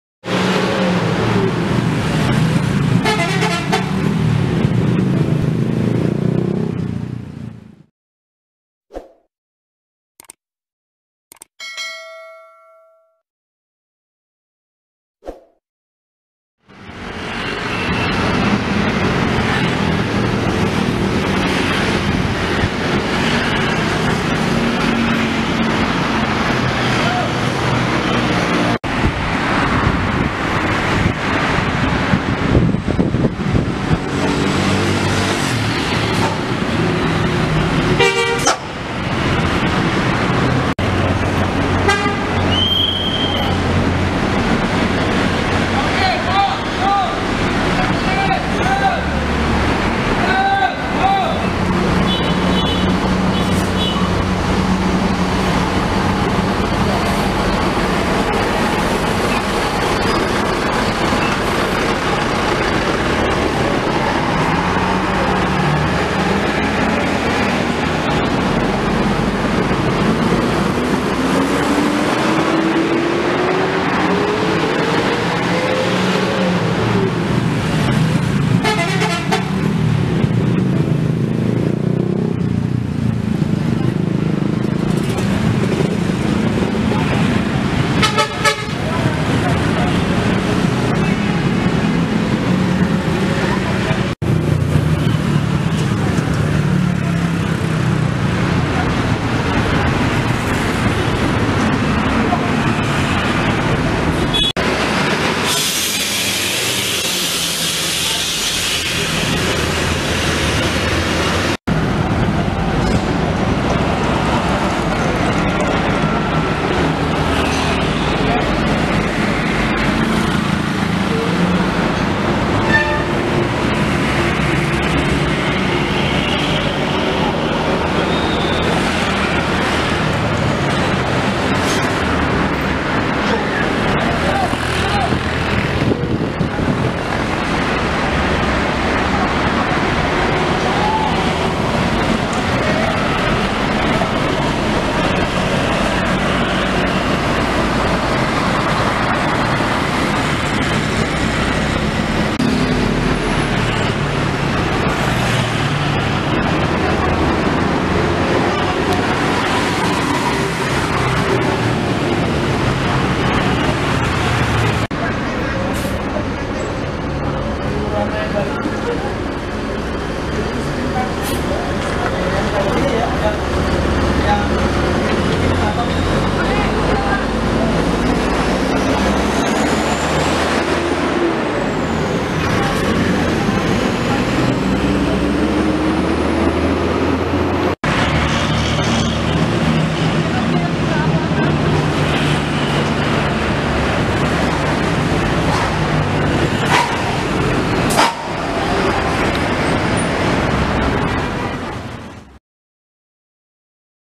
RING ROAD SELATAN JOGJA 😍 SULTAN PALUR CABANG JOGJA ❗❗
Kembali lagi di Channel Youtube Adi Sasongko

Video kali ini saya hunting di jalan lingkar (Ring Road) Jogja didepan agen Rosalia Indah. Bagaimana keseruannya? Ikuti videonya sampai habis yaa 😁

Terima kasih atas segala dukungannya 😊